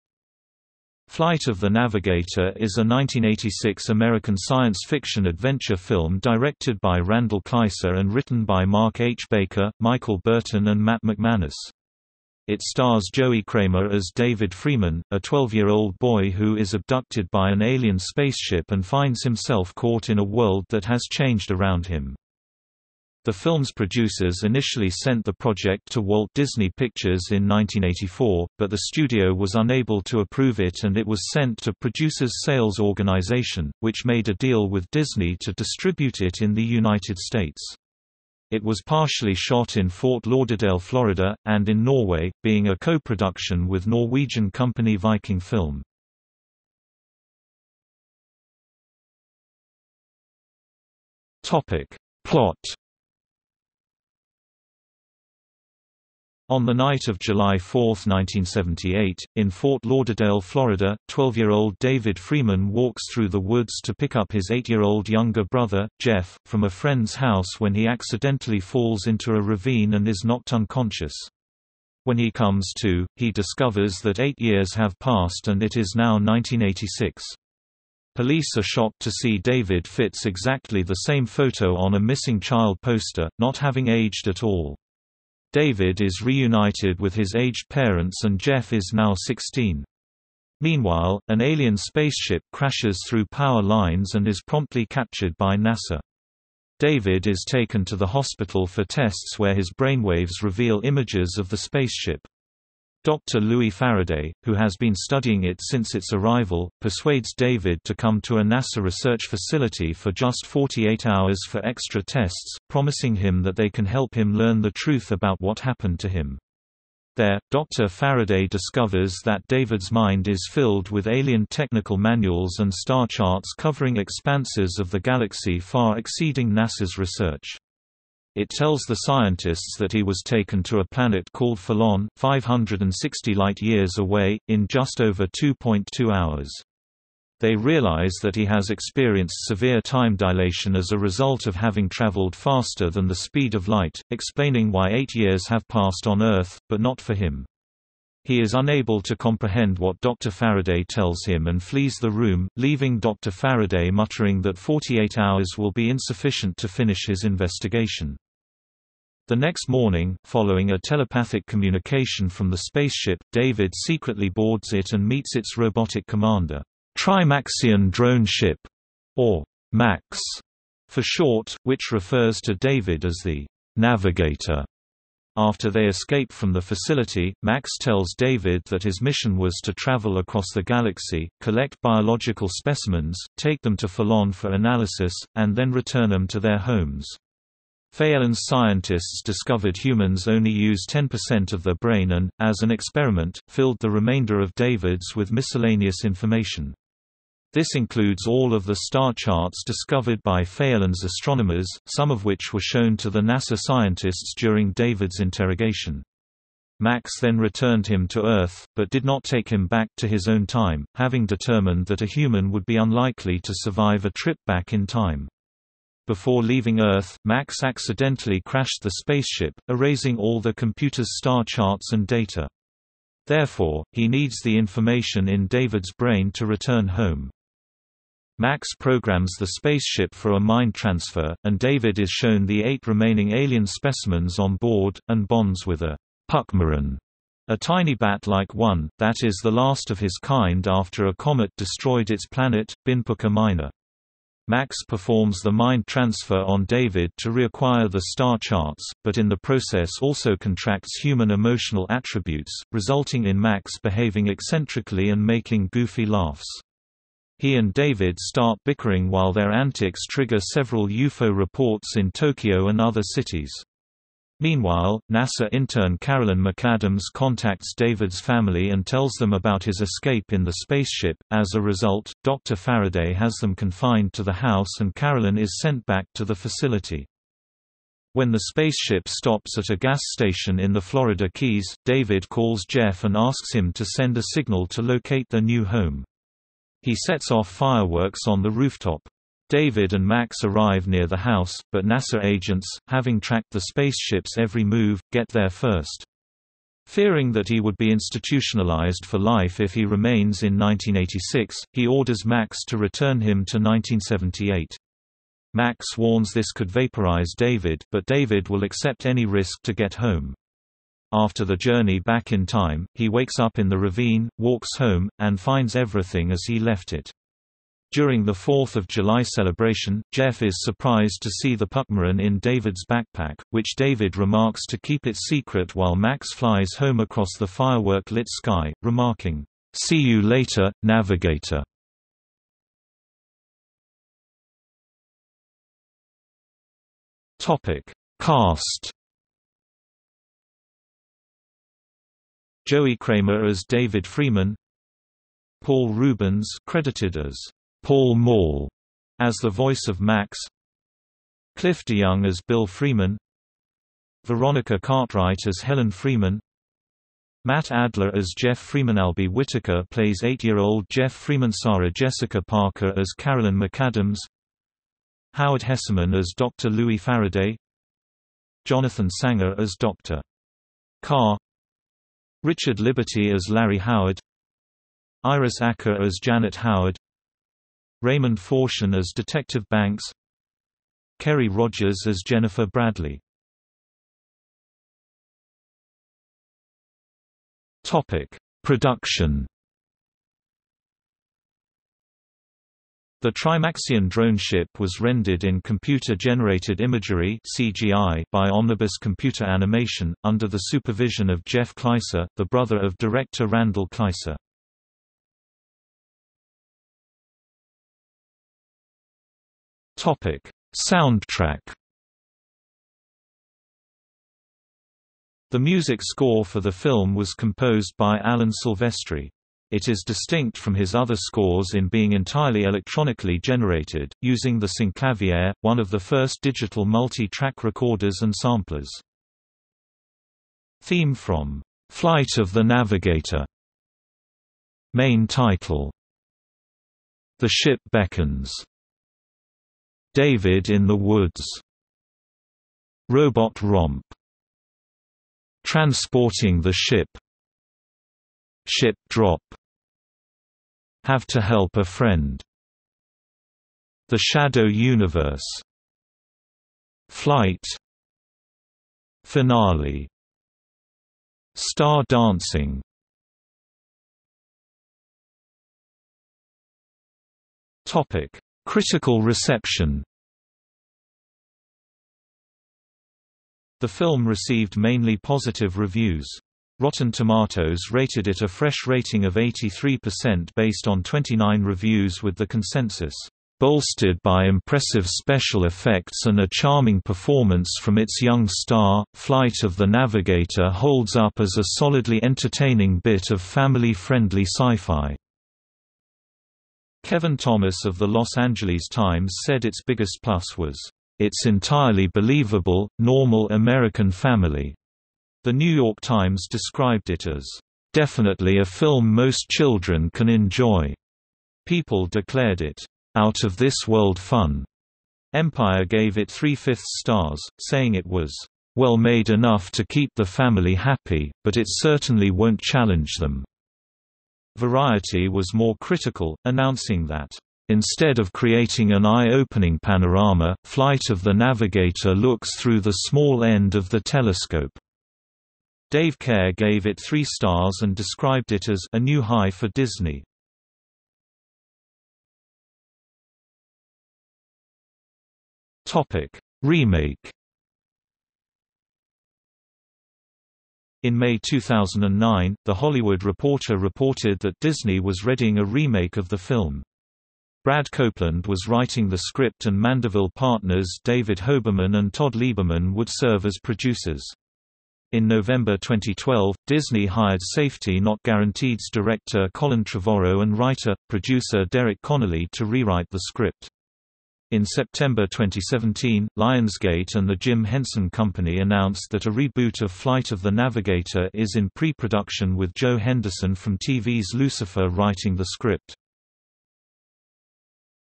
Flight of the Navigator | Wikipedia audio article
00:00:47 1 Plot
00:05:33 2 Cast
00:06:24 3 Production
00:06:46 4 Soundtrack
00:07:44 5 Critical reception
00:09:02 6 Remake

- Socrates

SUMMARY
Flight of the Navigator is a 1986 American science fiction adventure film directed by Randal Kleiser and written by Mark H. Baker, Michael Burton and Matt MacManus. It stars Joey Cramer as David Freeman, a 12-year-old boy who is abducted by an alien spaceship and finds himself caught in a world that has changed around him.
The film's producers initially sent the project to Walt Disney Pictures in 1984, but the studio was unable to approve it and it was sent to Producers Sales Organization, which made a deal with Disney to distribute it in the United States. It was partially shot in Fort Lauderdale, Florida, and in Norway, being a co-production with Norwegian company Viking Film.